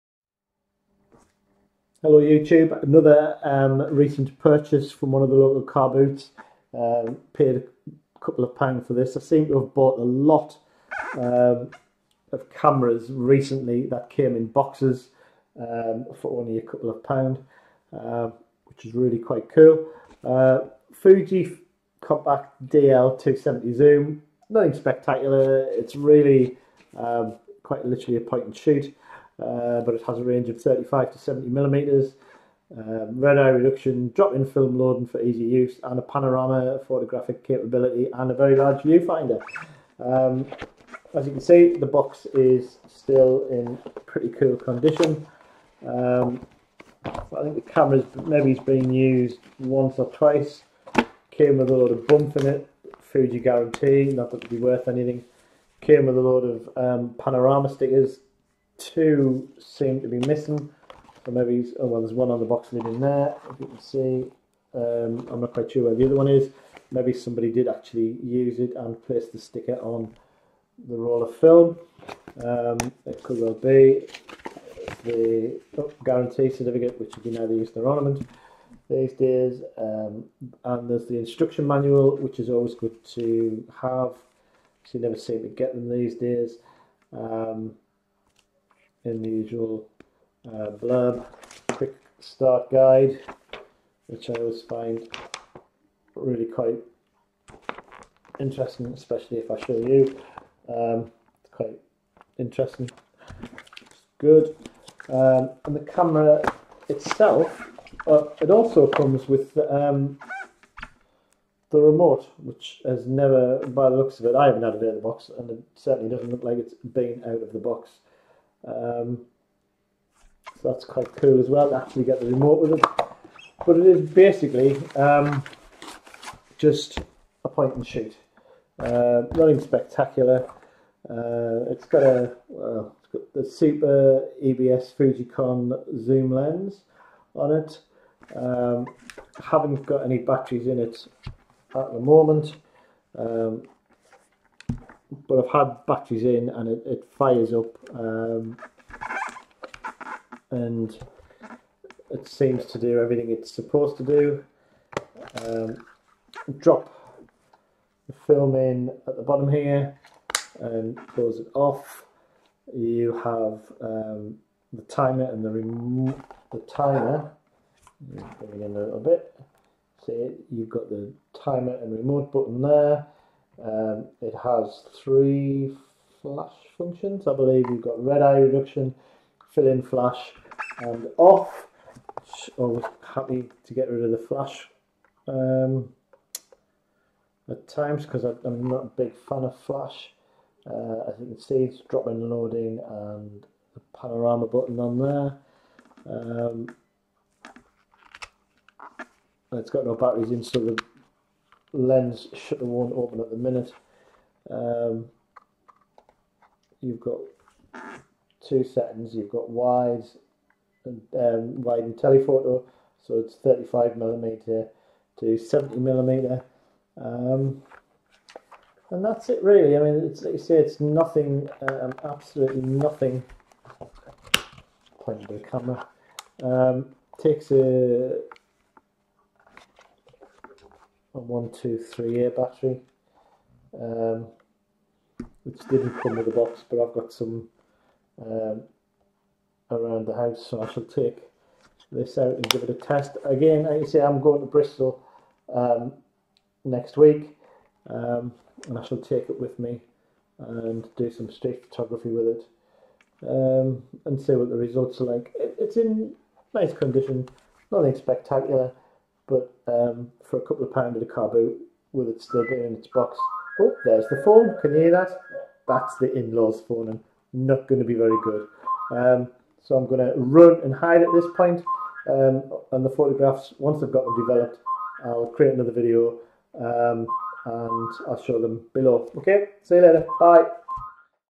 Fuji DL270 Zoom point and shoot
A recent purchase at a local carboot. Nothing more than a simple point and shoot camera but for £2 it's worth picking up. The test results from the camera will be posted here in another video next week and on my Flickr account and my blog, the links are below